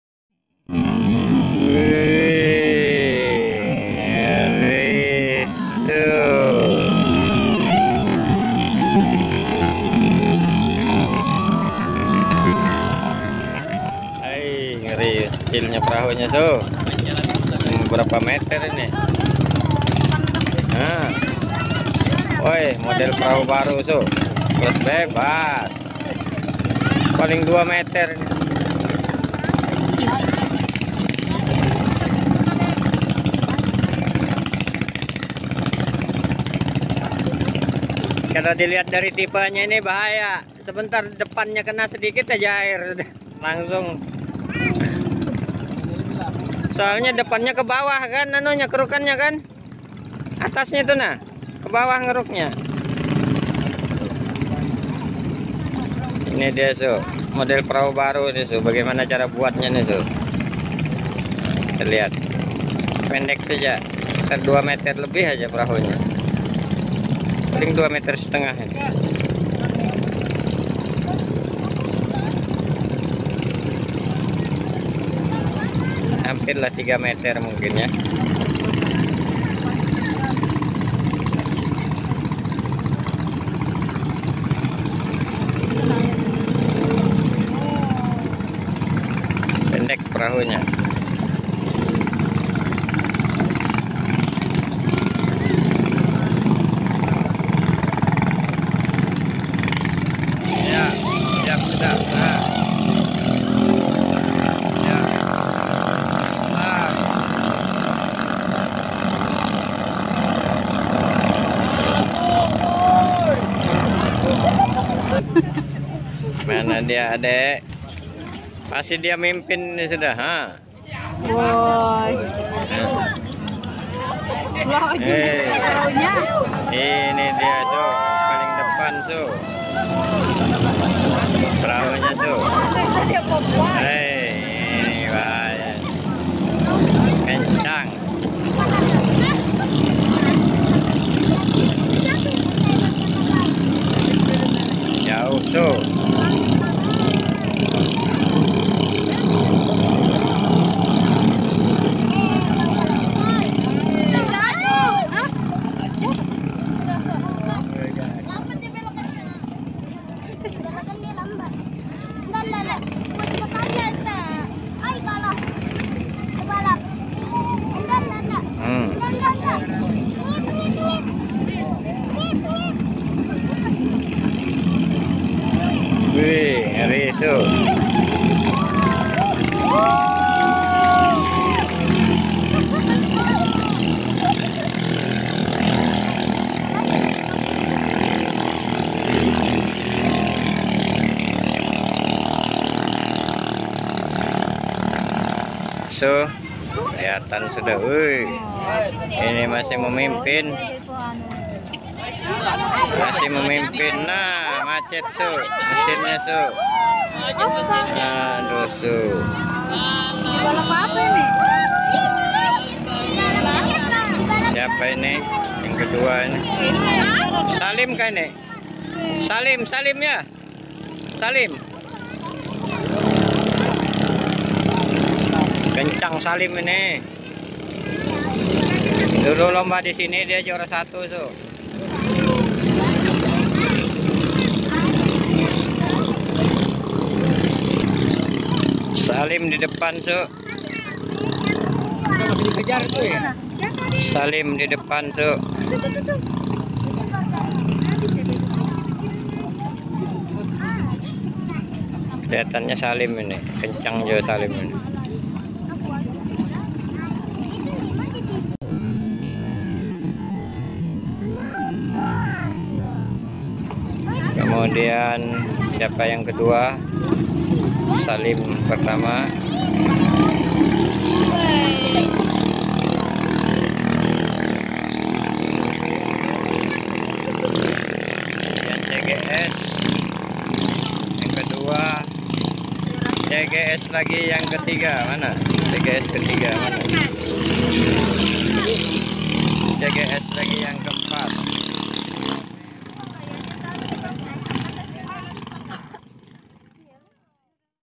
world's smallest boat - perahu terkecil sedunia ikut balap ketinting di sambaliung- akmal mantaritip
akmal penjoki handal asal palinjau mantaritip kecamatan sambaliung kabupaten berau kalimantan timur indonesia.